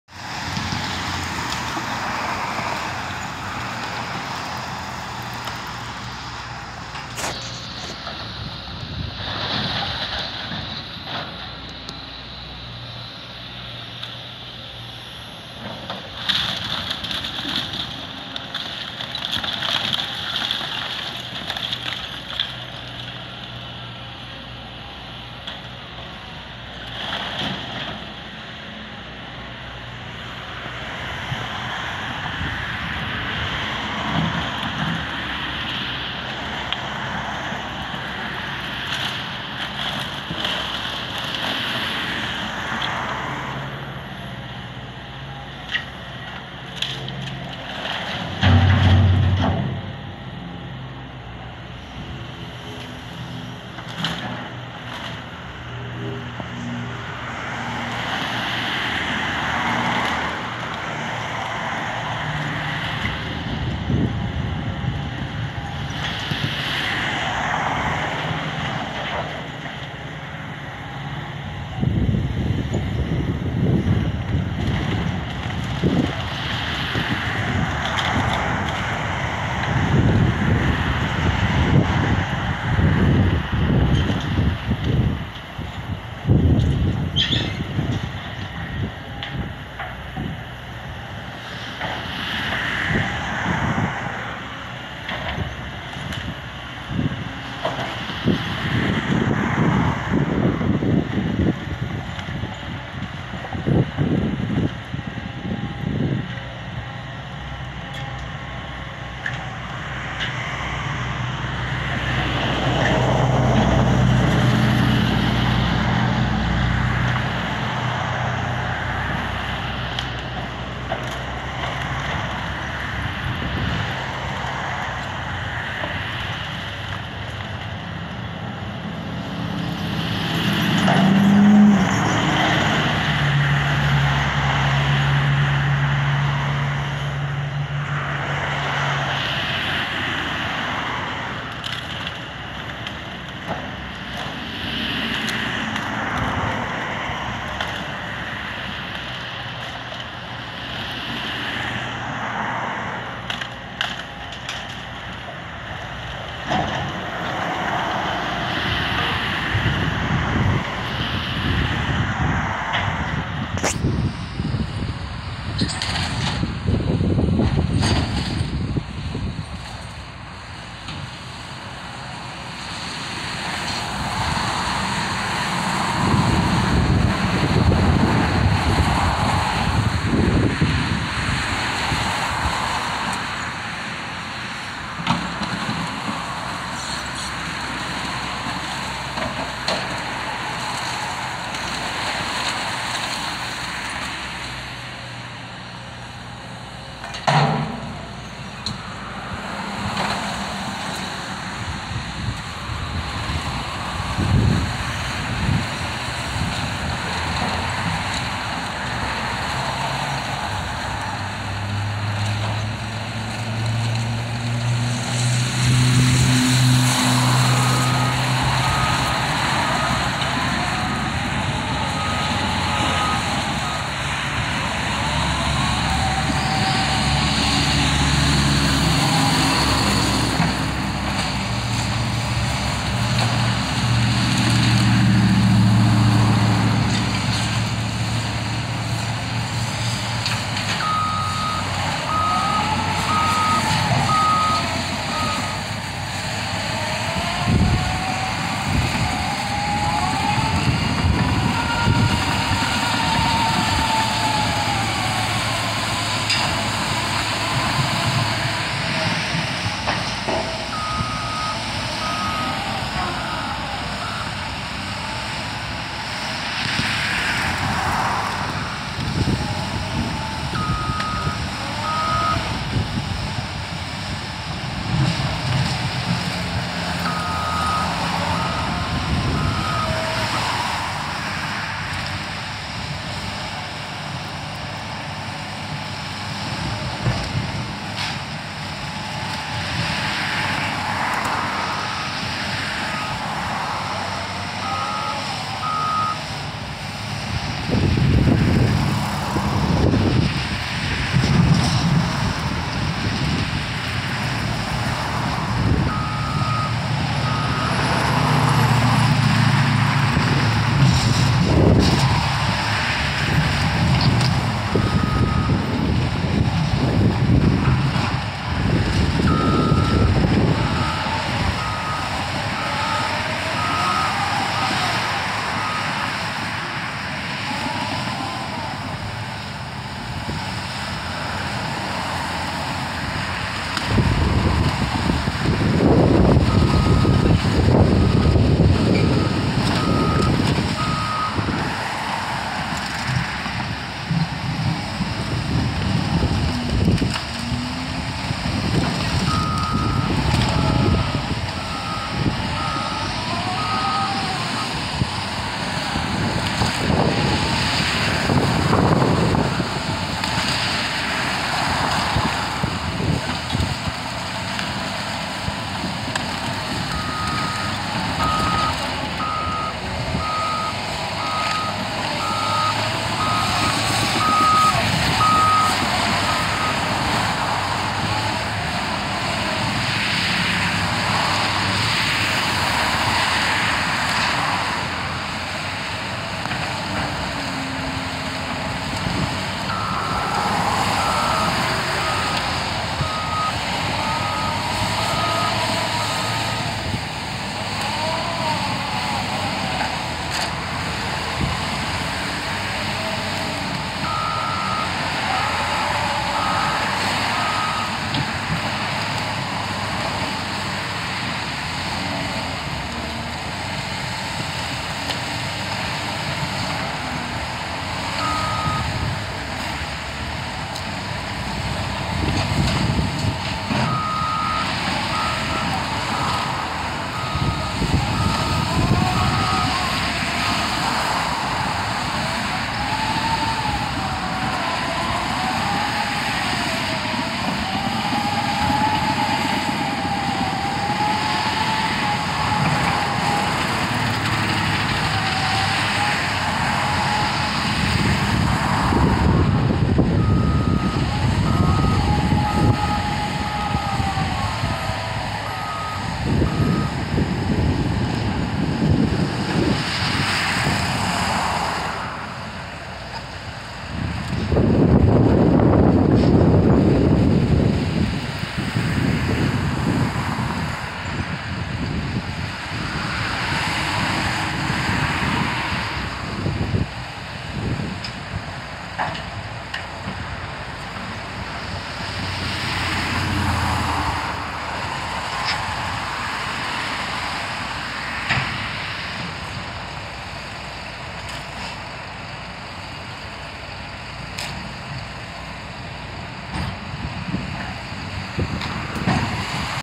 CAT 953D Track Loader & Deere 130G Excavator Work Together to Demolish A House
In this video, I caught a CAT and a Deere working well together in the wild to move some recently demolished house debris into a dump truck. This video captures all the machines working together with real sound and picture, and nothing edited in. I hope you enjoy.